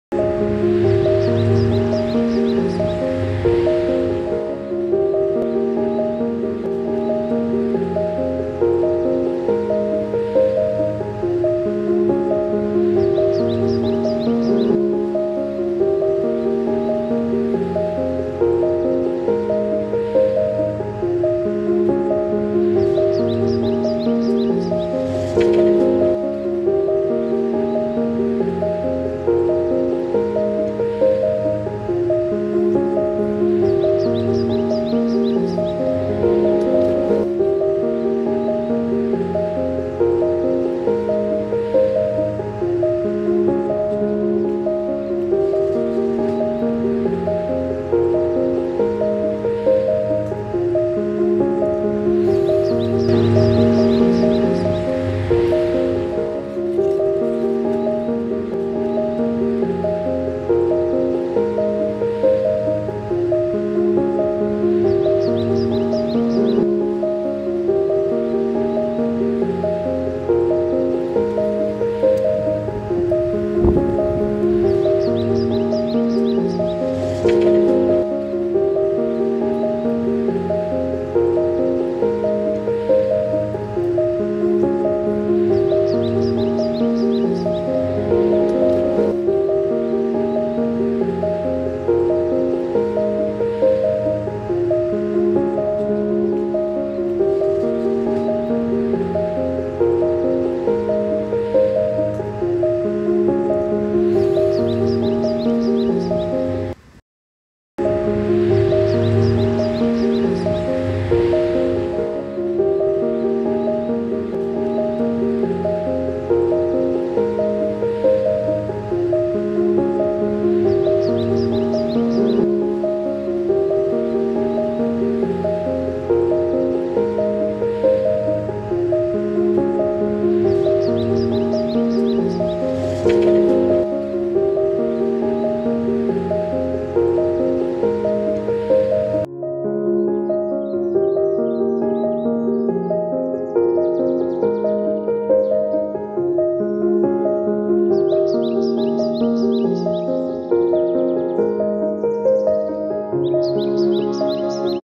BBQ fish with Vegetables in the garden/Cá nướng rau vườn cuốn bánh tráng chấm mắm ớt.🤗
Bbq fish with Vegetables in my garden.